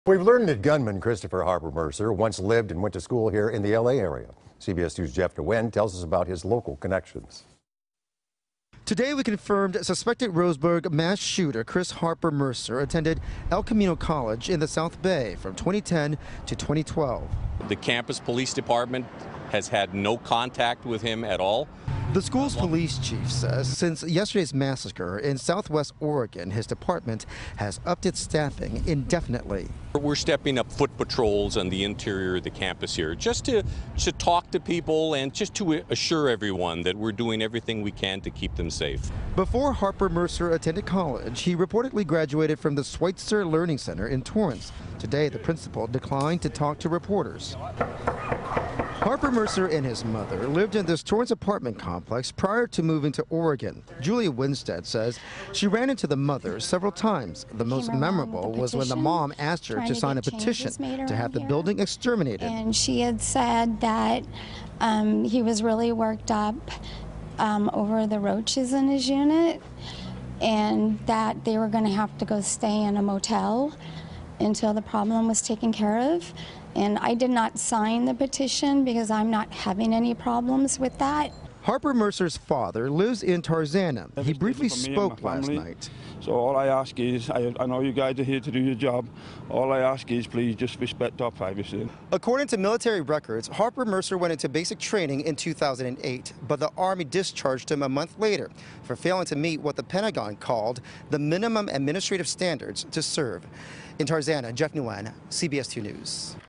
Oregon Mass Murderer Had Many Ties To Southland
Chris Harper Mercer, accused of killing at least nine people during a rampage in Oregon, had many ties to the southland -- both school and family. Jeff Nguyen reports.